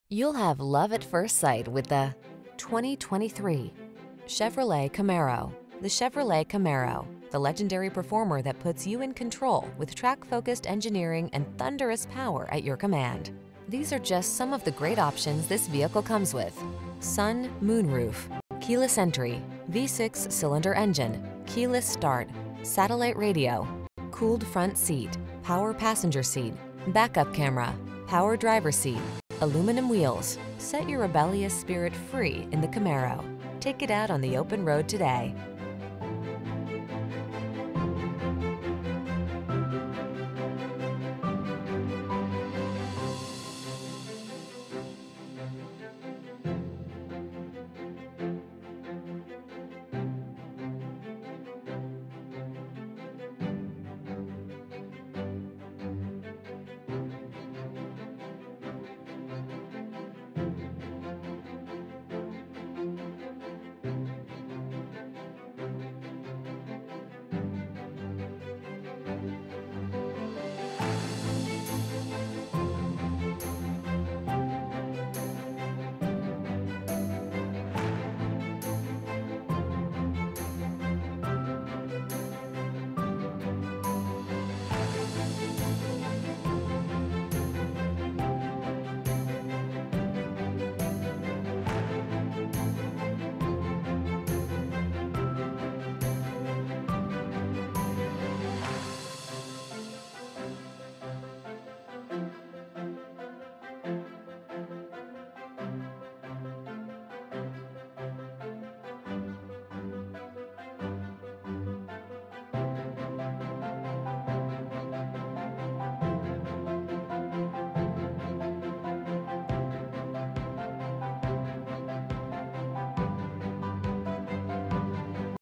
2023 Chevrolet Camaro Owasso, Tulsa, Claremore, Pryor, Broken Arrow, OK N816400
Summit White New 2023 Chevrolet Camaro available in Owasso, Oklahoma at Suburban Chevrolet. Servicing the Tulsa, Claremore, Pryor, Broken Arrow, OK area.

Suburban Chevrolet
1300 S Lynn Riggs Blvd